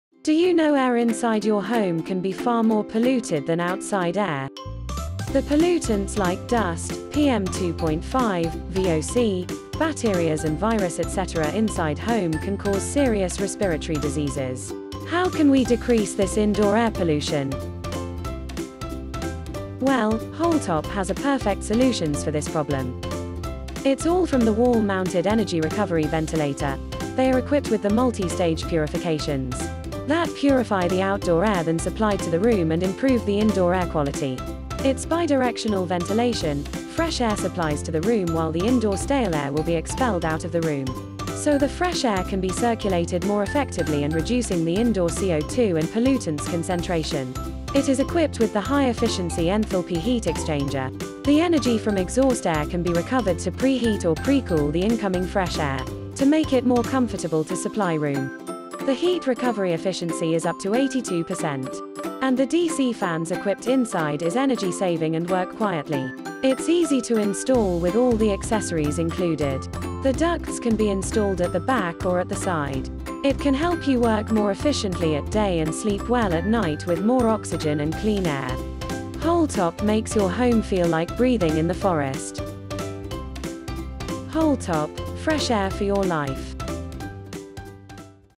Improve Indoor Air Quality with Holtop Wall Mounted Purification Type Energy Recovery Ventilator
Holtop mounted heat energy recovery ventilator brings clean fresh air to your life. It can remove the air pollutants such as dust, PM2.5, VOC, bacteria and virus, etc. The installation is so convenient that you don't need to do ducting work over the ceiling. Just dill two holes on the wall and hang the machine with complimentary vents accessories.